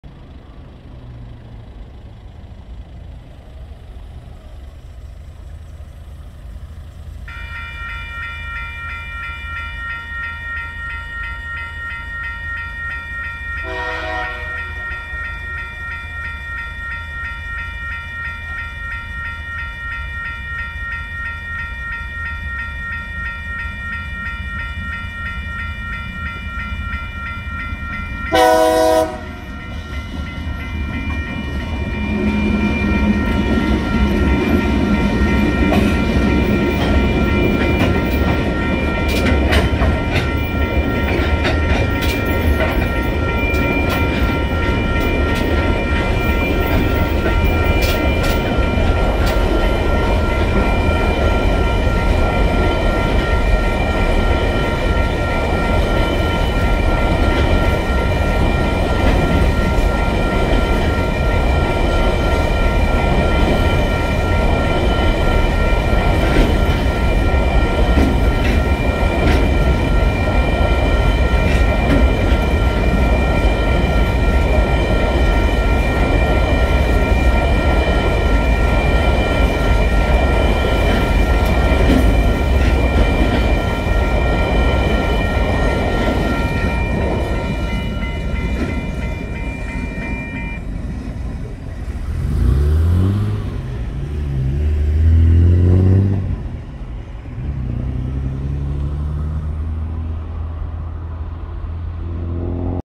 TasRail TR03 TR12 #31 train crossing Leighlands Road
TasRail #531 train lead by TR03 crossing Leighlands Road departing Western Junction heading to Conara Junction.

21:05 Thursday the 4th of November 2021.